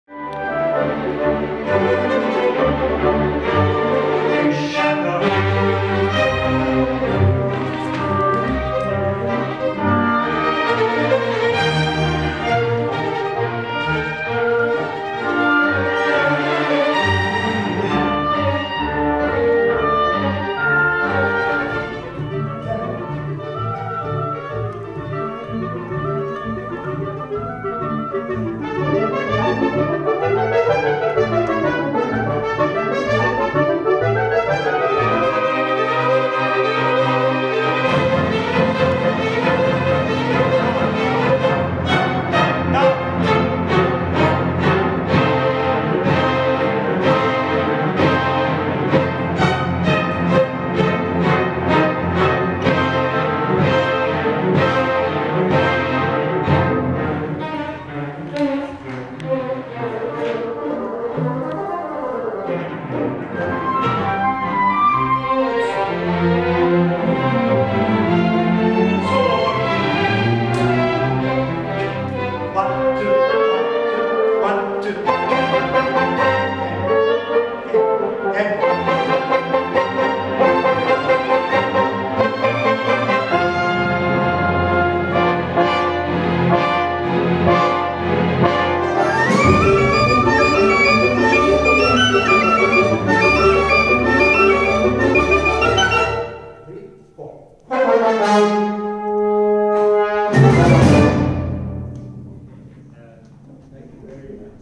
New Manhattan Sinfonietta Rehearsal Day 1
With Maestro Gurer Aykal - New Manhattan Sinfonietta had it's first day rehearsal.